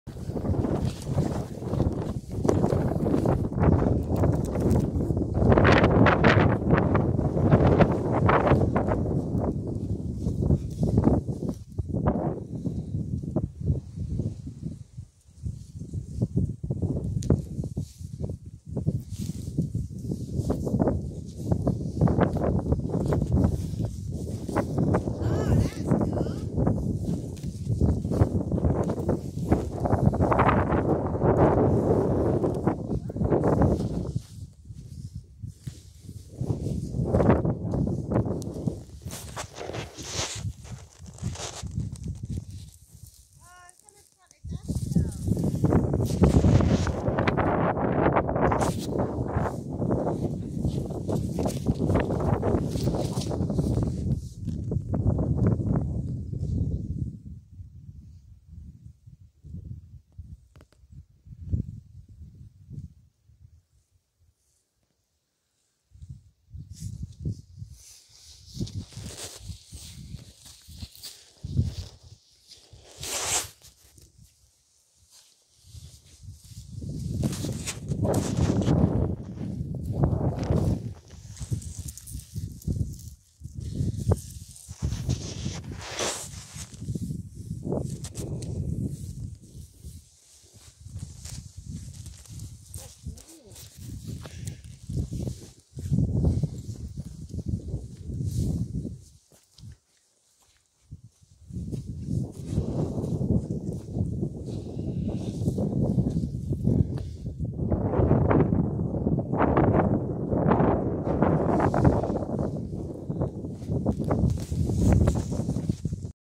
NC500. WW11, USAAF Liberator Crash Site. Gairloch.
A crash site, ruins and memorial to the fallen heroes of WW11 who sadly crashed on their final journey home after the war.